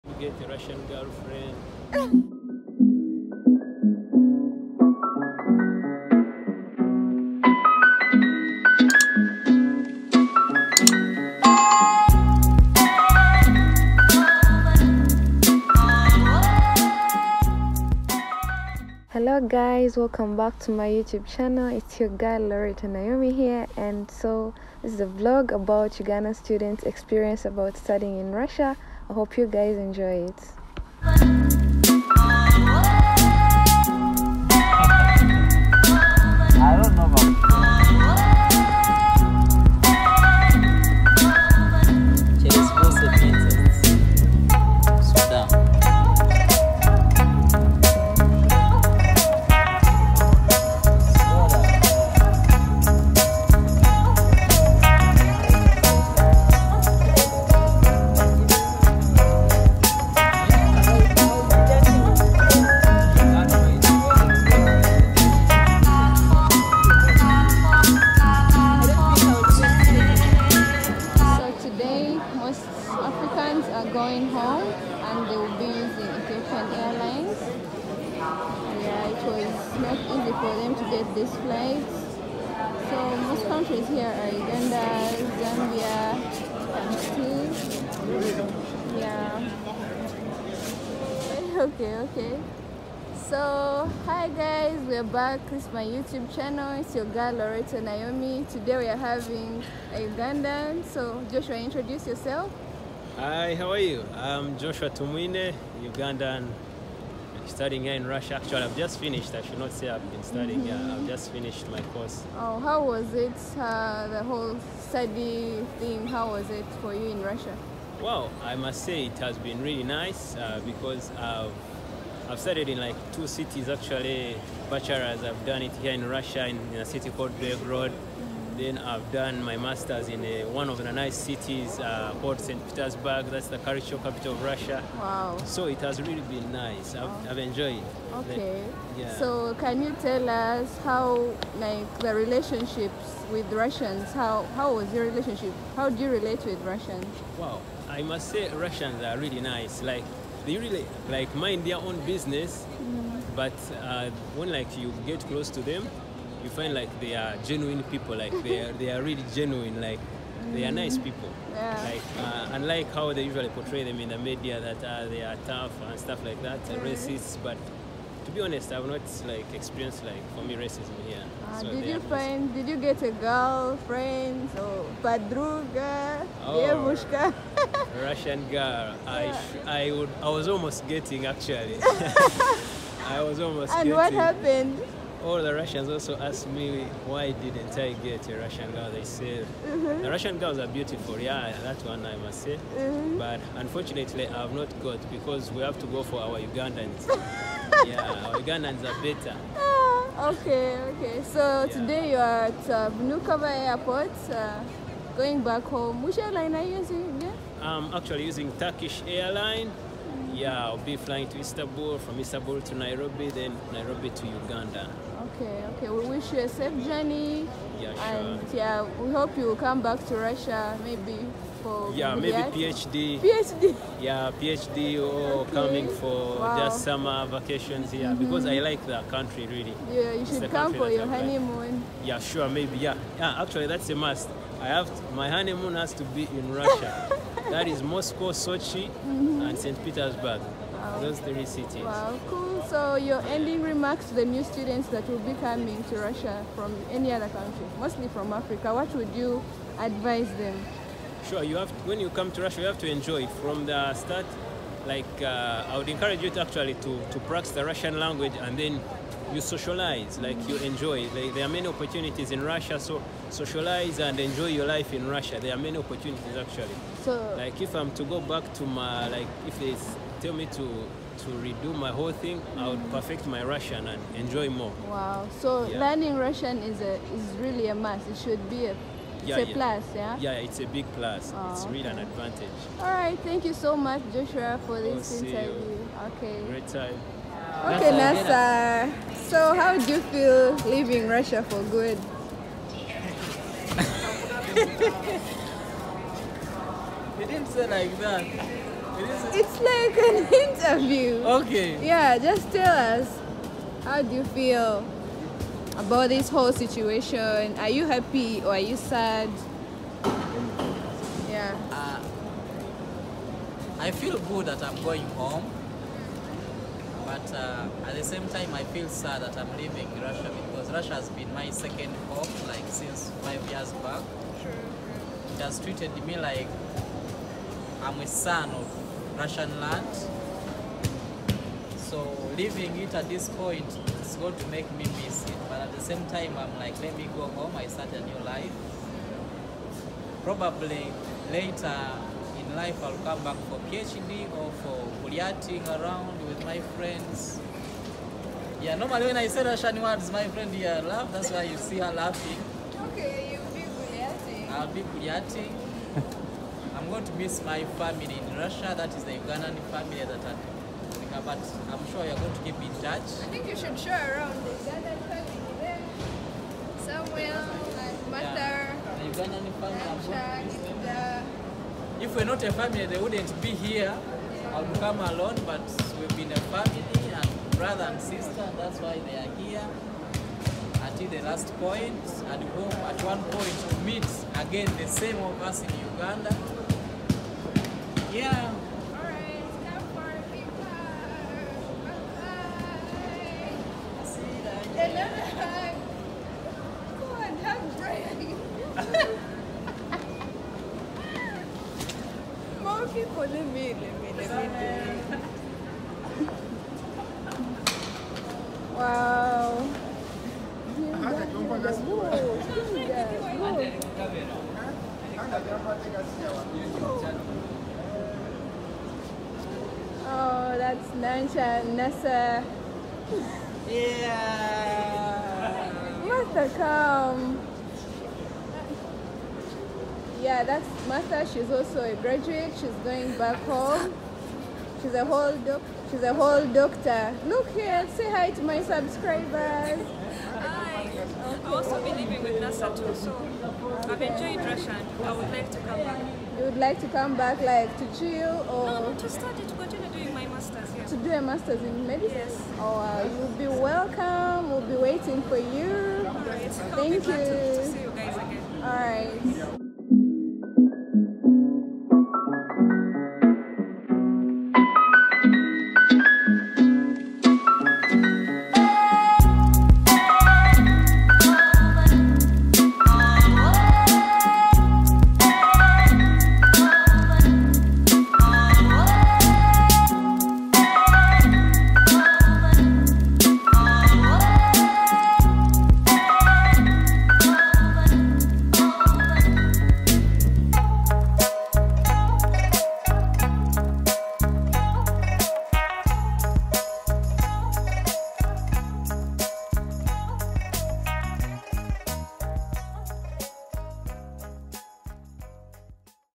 Ugandan Student Graduates Tell Their Experiences About Studying In Russia. Watch Till The End
The 2020 graduates had a few things to tell us about their life in Russia and what they will miss most about this country .